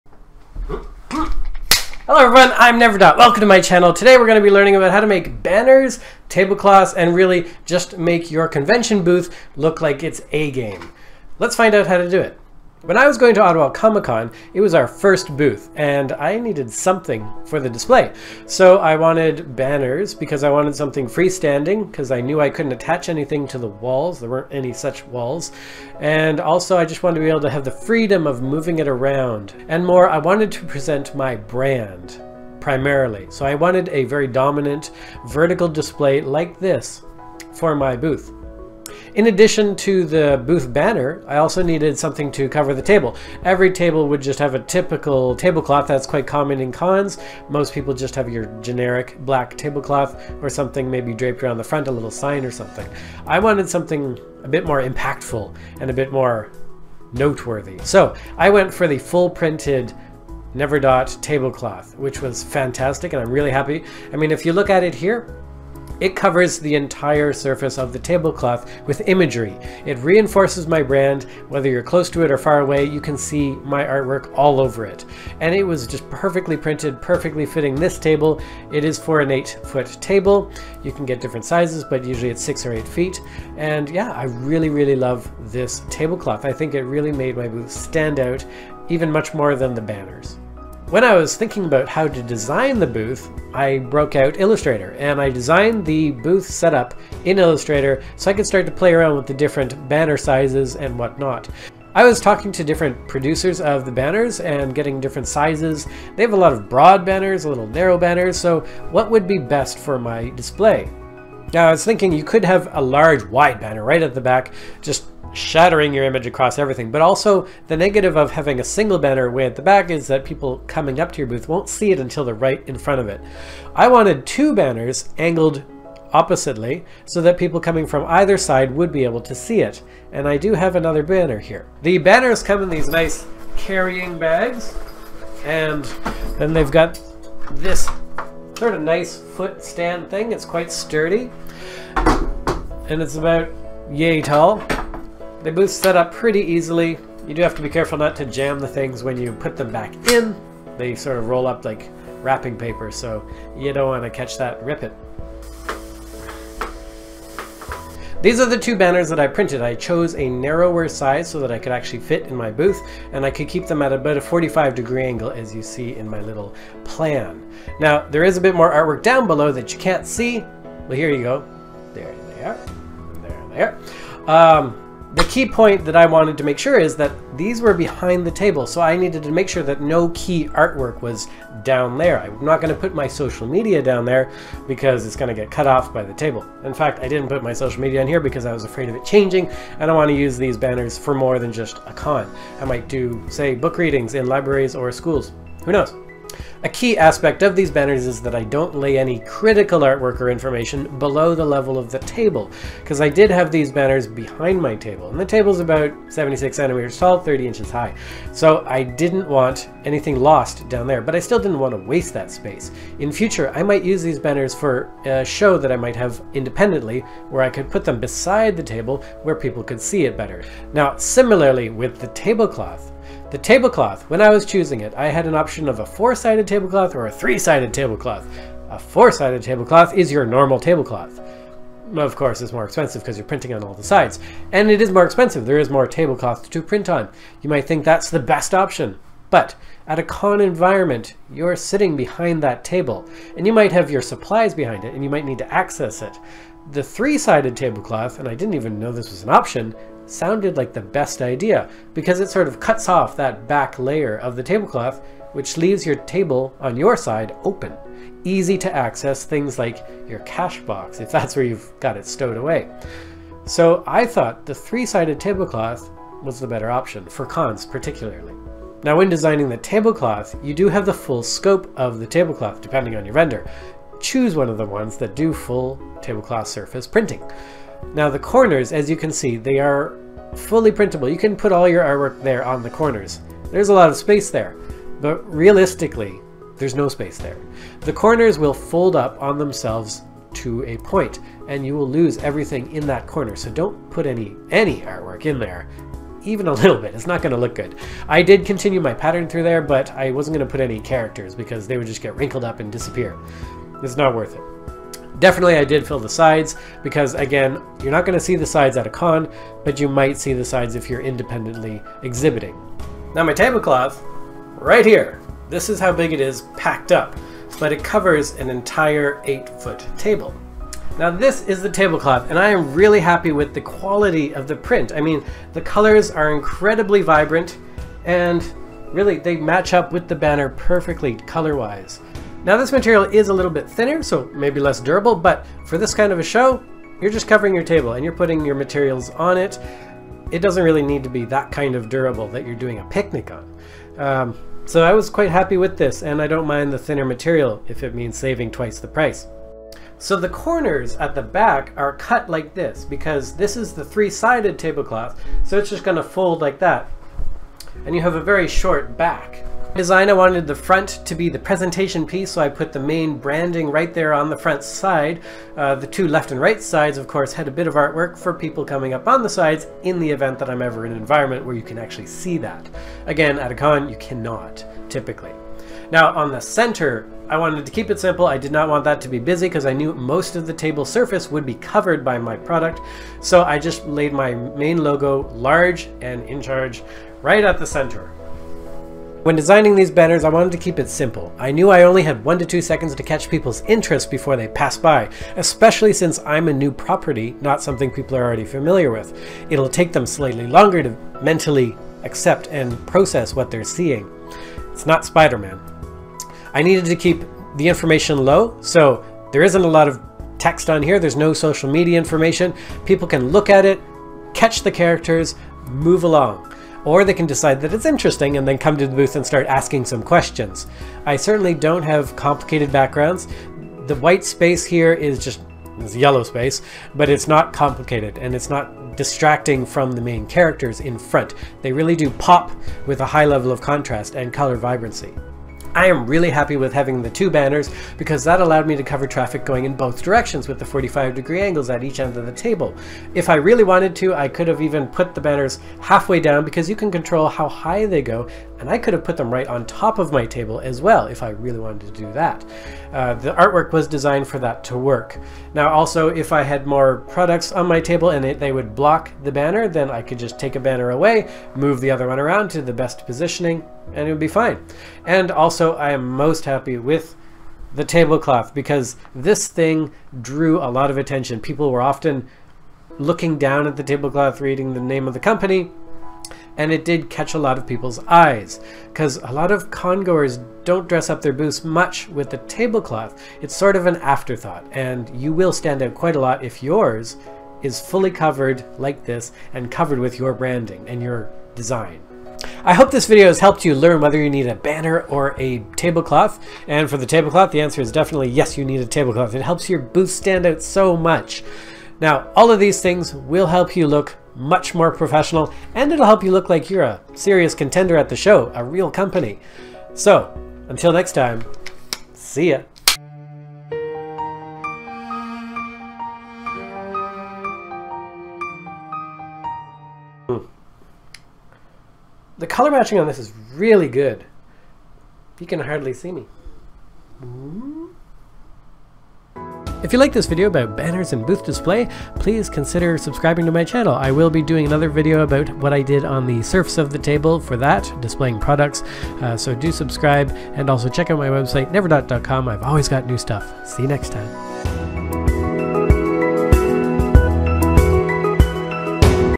How to make a booth display
Come with me to see how I made my banners and amazing tablecloth for my first comic con!

The banners and tablecloth featured in this video were created by BannerBuzz ➠

Music ➠ Ricardo Alvarez